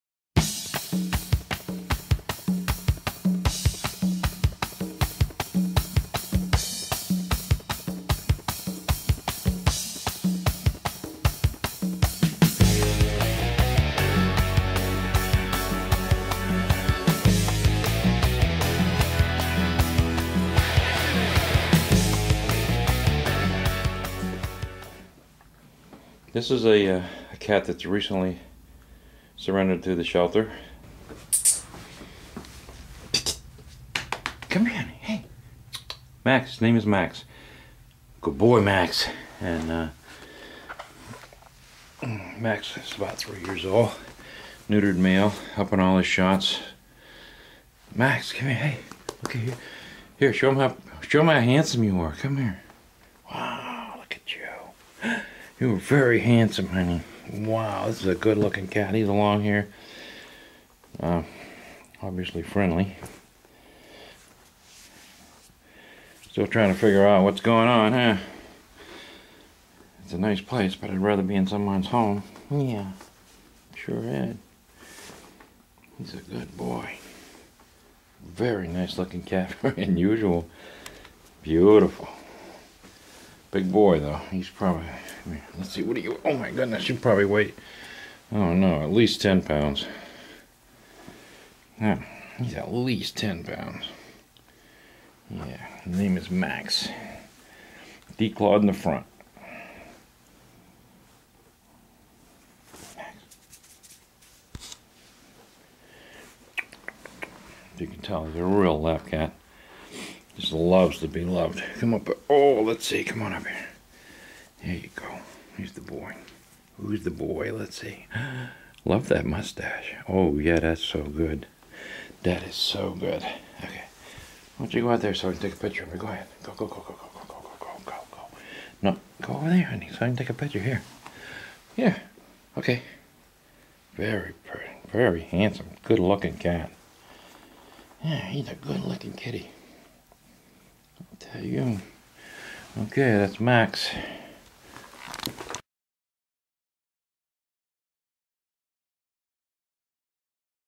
M A X the cat ADOPTED !
Max is a male declawed cat at the shelter.
UP DATE! Max was adopted 4-5-08
Please always get your pets from a shelter or breed rescue.